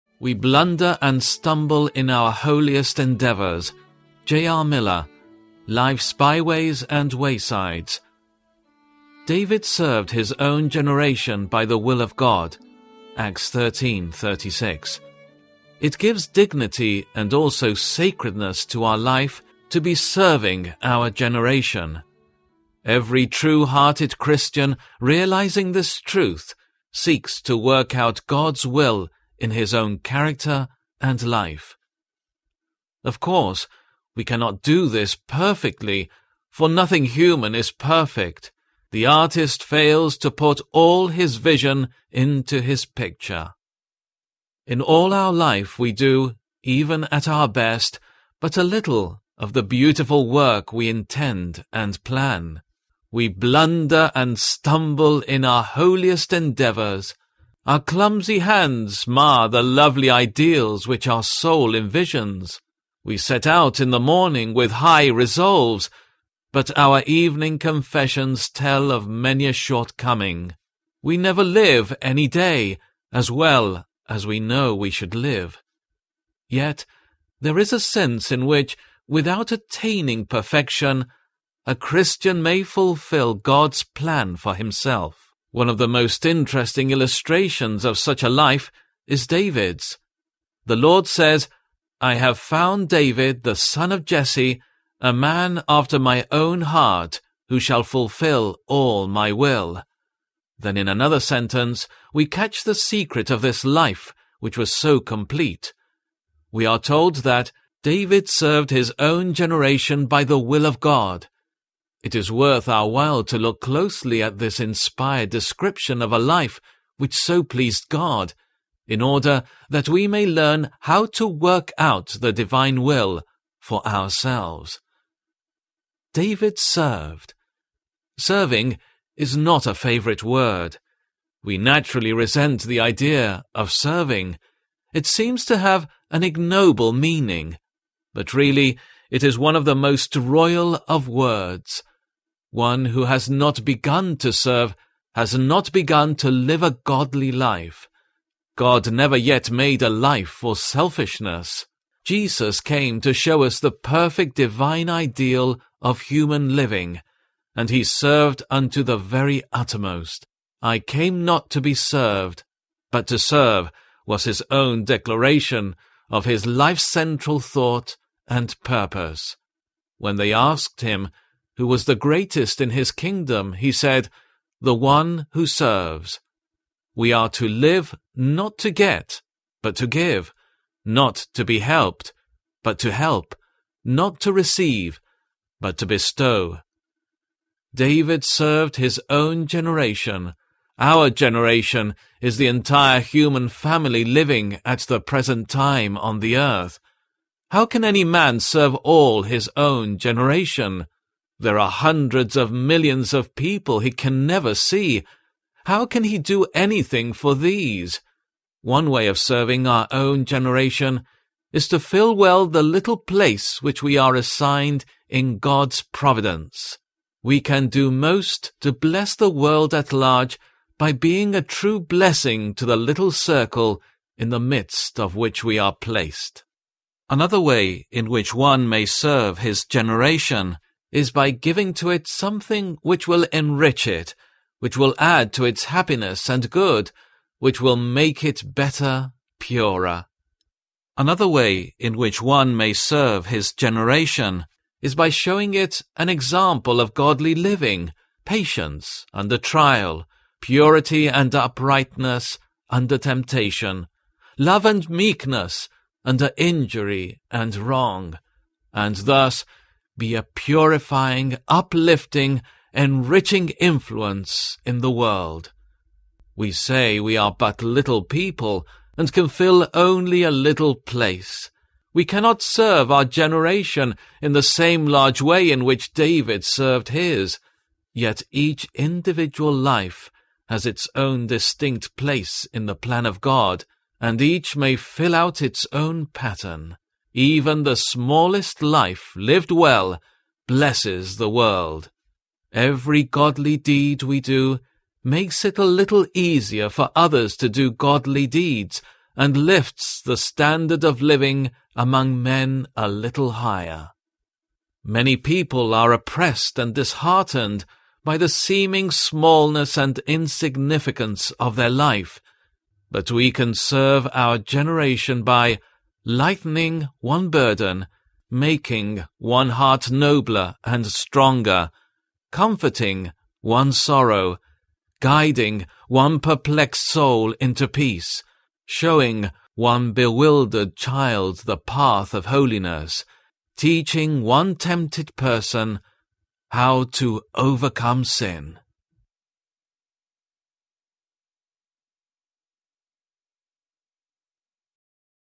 We Blunder and Stumble in our Holiest Endeavors - J. R. Miller (Christian devotional)
J. R. Miller - We Blunder and Stumble in our Holiest Endeavors

A Treasury of Ageless,
Sovereign Grace,

Acts 13:36 "Now when David had served God's purpose in his own generation, he fell asleep; he was buried with his ancestors and his body decayed.

James Russell Miller was born on March 20, 1840 at Frankfort Springs, Pennsylvania and died on July 2, 1912. Besides authoring over 80 books, booklets, and pamphlets, he was the Editorial Superintendent of the Presbyterian Board of Publication and a very active pastor in a succession of churches.

The crucible of his education was his service with the United States Christian Commission, an agency set up to minister to the troops, during the civil war. When the war ended he completed his theological studies and was ordained and installed on September 11, 1867. On June 22, 1870, when he was thirty, he married Miss Louise E. King.

The end of life on earth came without warning on the afternoon of July 2, 1912. JR's wife, Louise Miller, and their only daughter, Mary Wanamaker Miller (Mrs. W.B. Mount), were present, but it was impossible to summon the sons — William King Miller and Russell King Miller. One moment he seemed to be resting quietly; the next he was at rest.

He was one of the best selling Christian authors of his era. His books had a total circulation of over two million copies during his lifetime and in 1911 the Presbyterian Board of Publication, under his direction, published over 66 million copies of its periodicals.